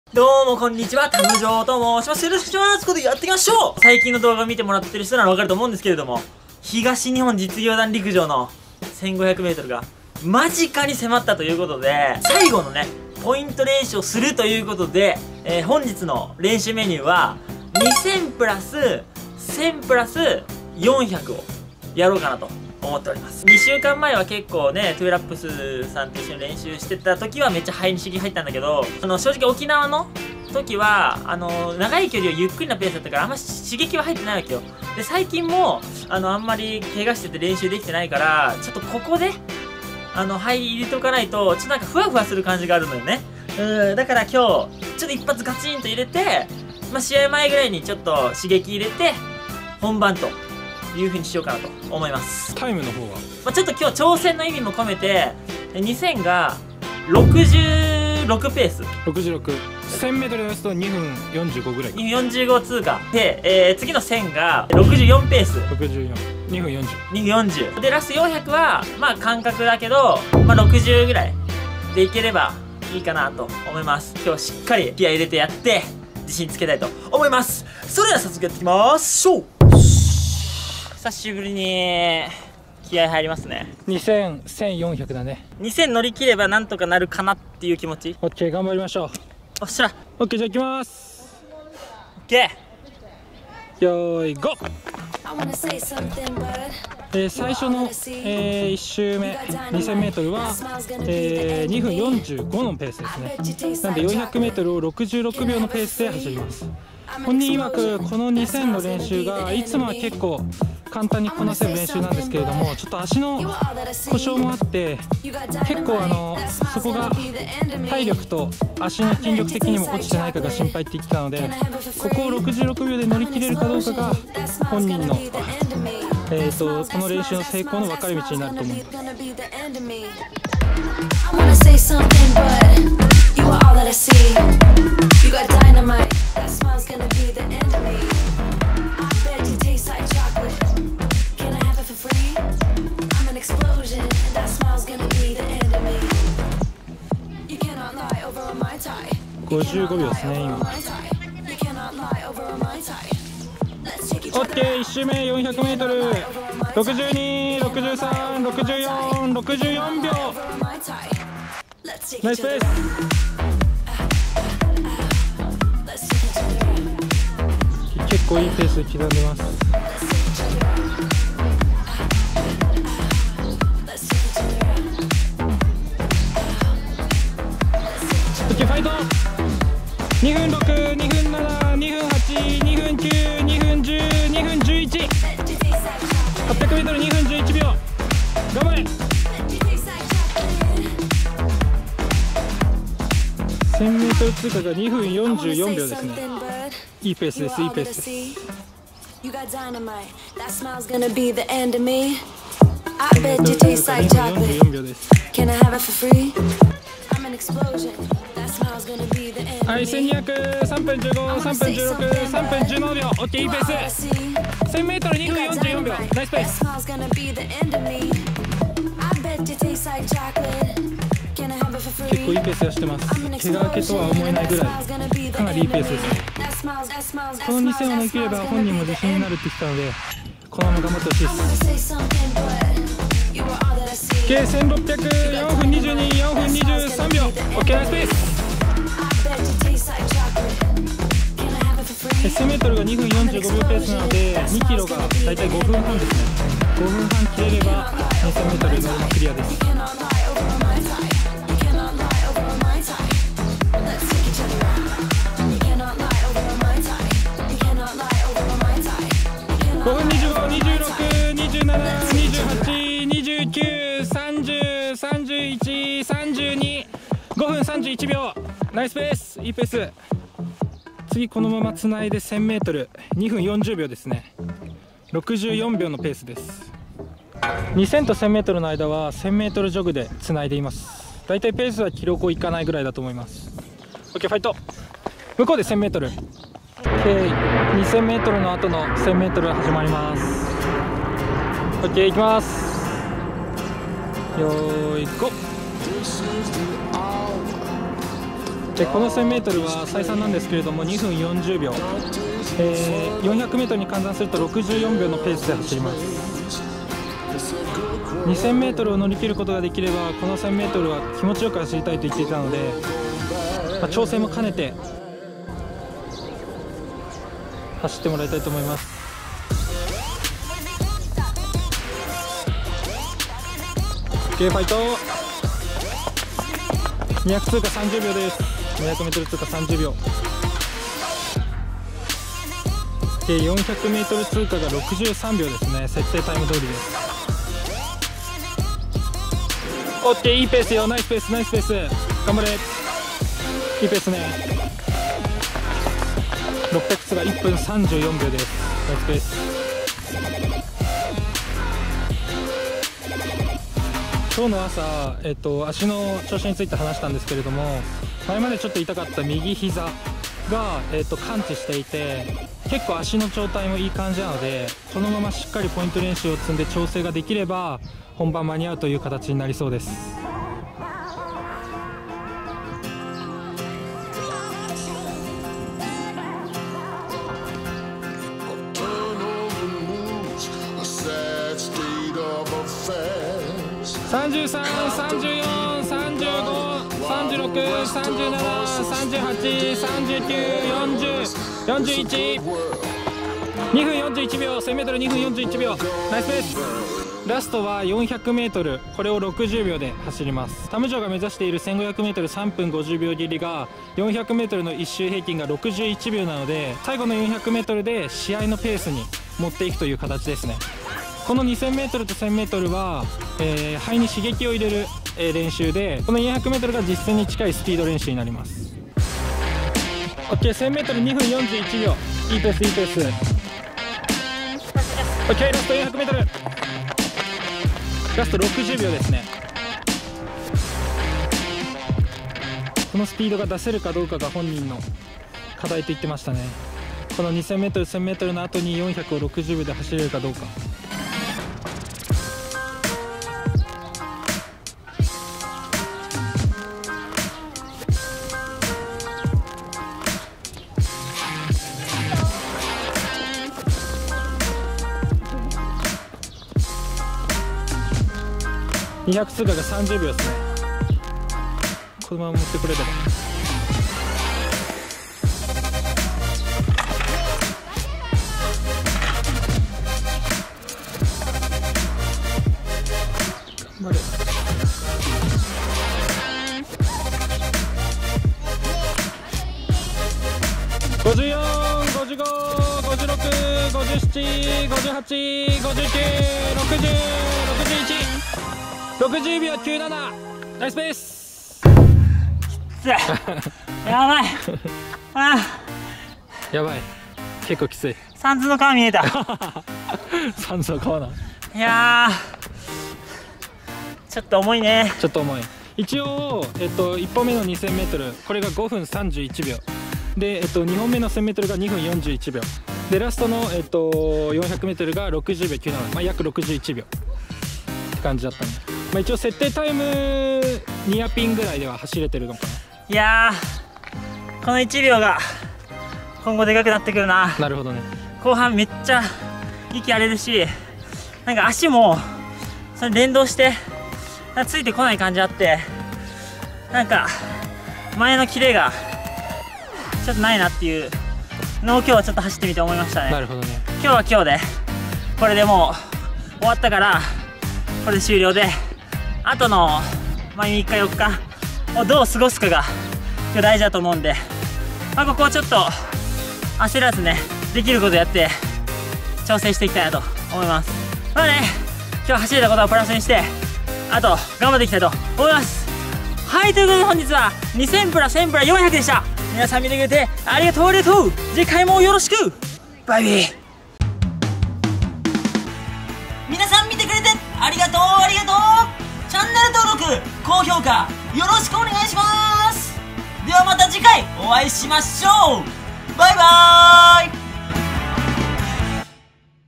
怪我明けのガチ練習で出走を決める！東日本実業団陸上直前2000m+1000m+400mインターバル練習【陸上競技】【1500m】
たむじょー【Tamujo】

たむじょー【ゲーム実況】

たむじょーYouTube【公式】

🟢note
たむじょー🏃‍♂️

たむじょー🏃‍♂️

たむじょー(Tamujo)